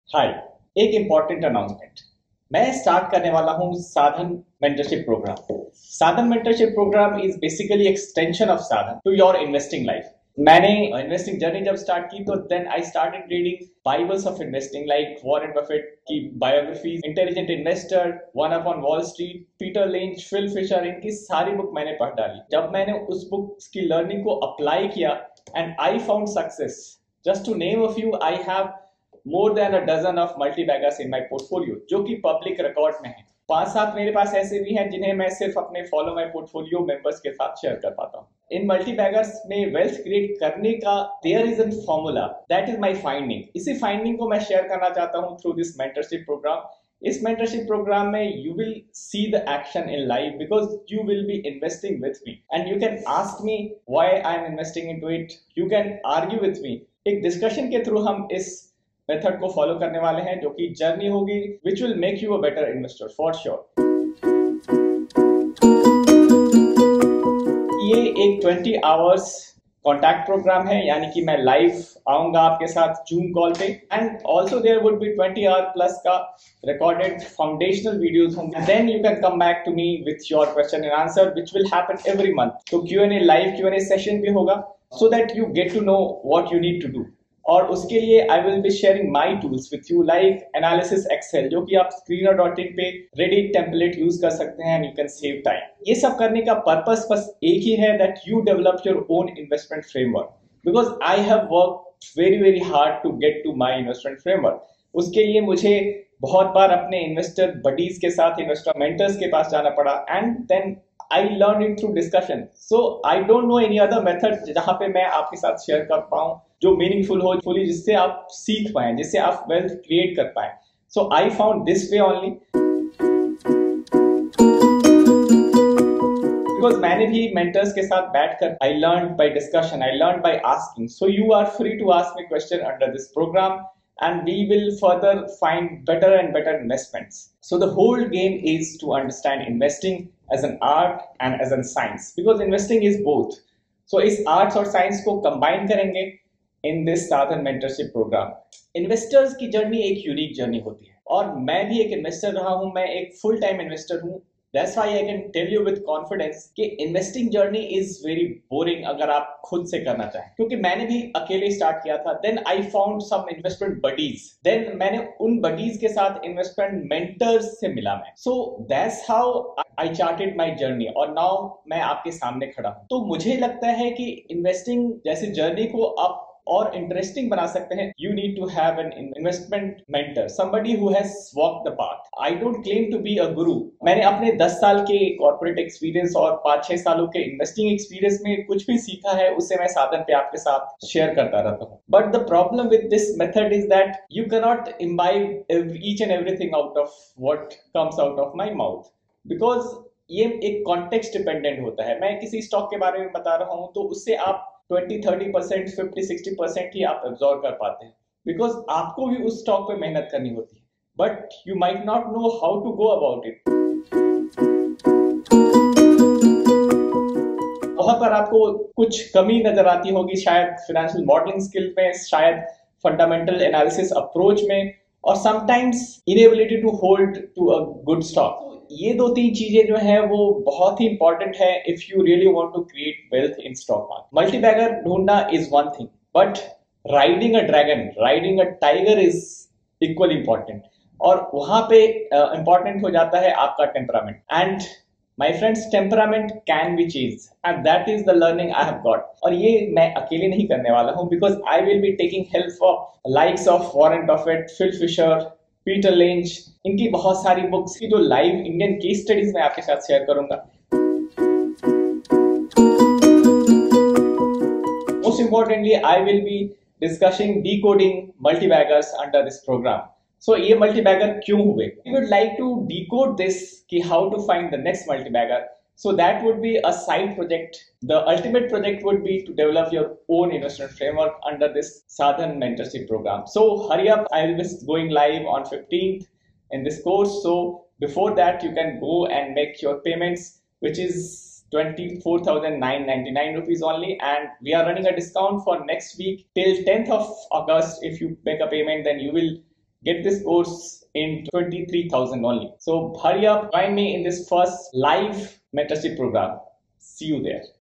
My Path to 20 MULTIBAGGERs | Sharing My Investment Secrets| Investor Mentorship Program
How I found my way to 20 multibaggerstock in my investing | I am sharing my Investment secrets in a structured Investor Mentorship Program. Check out the details here